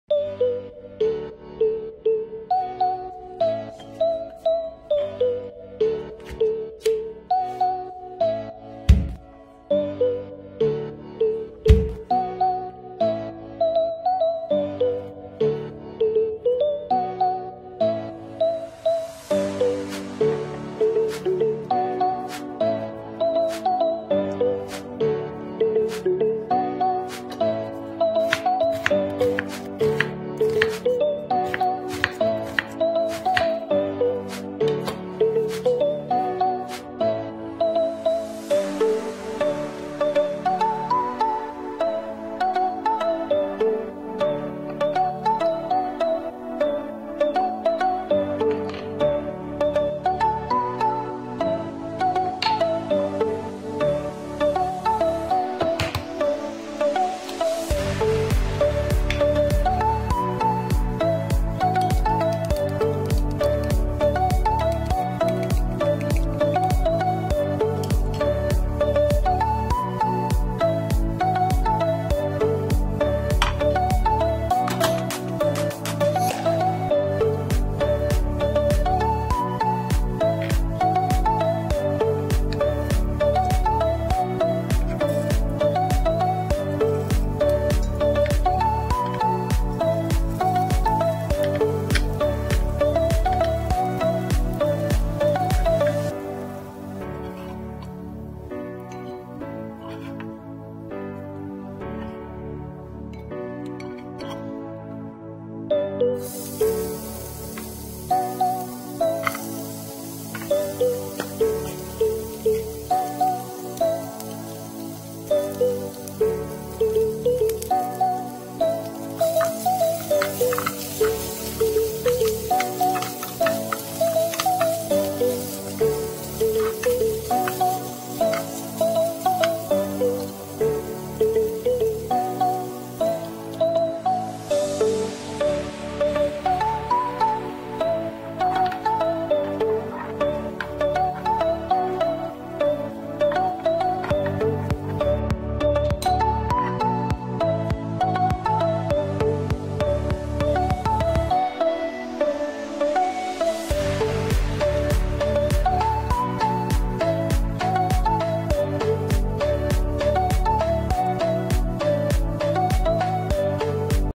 크래미로 이렇게 만들어 드세요!ㅣ태국 안가도 되는 맛이에요😊
크래미와 카레로 집에서 간단하게 즐길 수 있는 푸팟퐁커리입니다 :)